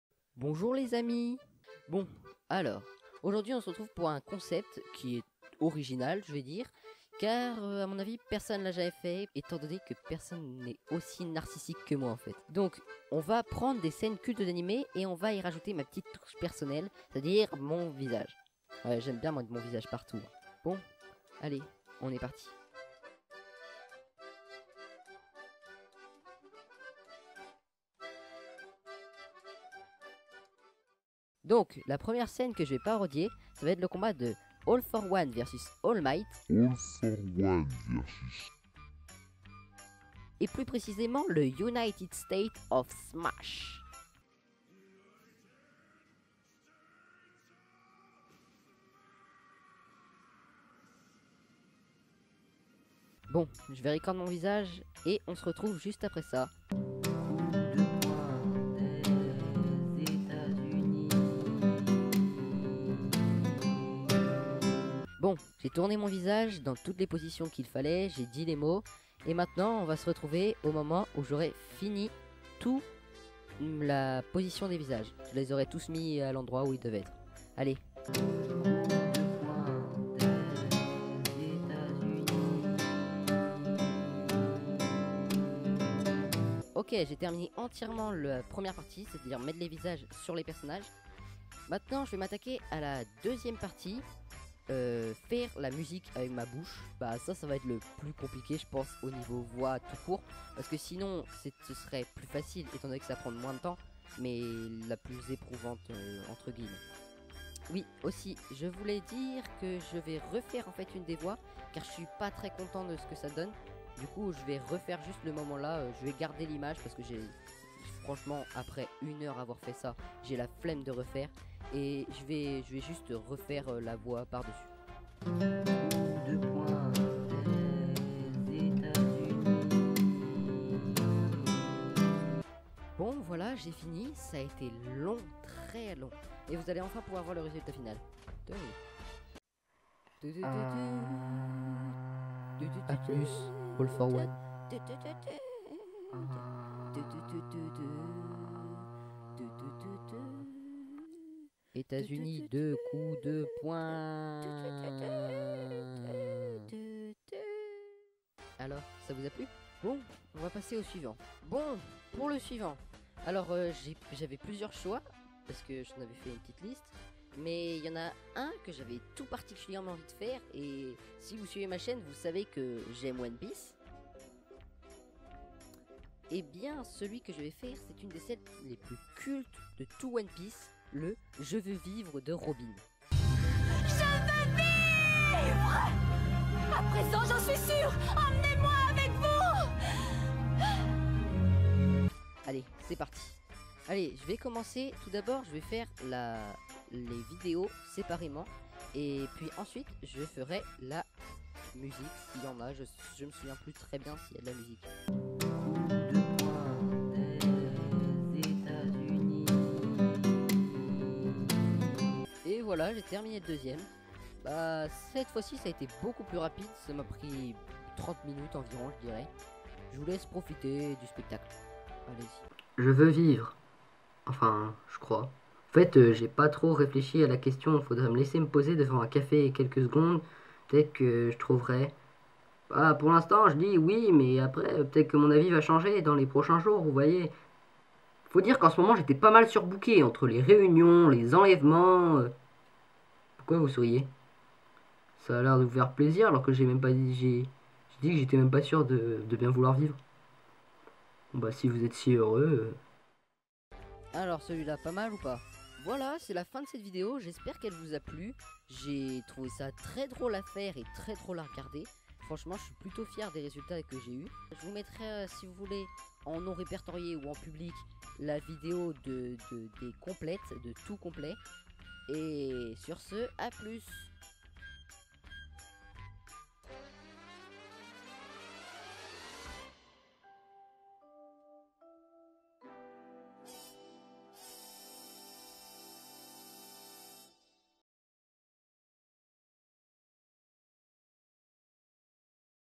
Ces moments d'animes cultes... à ma façon
Bonjour, bonjour, bonjour,
Aujourd'hui, je vais refaire des moments cultes d'anime mais avec mon visage.

J'espère que la vidéo vous plaira et sinon tant pis.
(๑✧◡✧๑) ╰(‘ω’ )╯ (๑✧◡✧๑)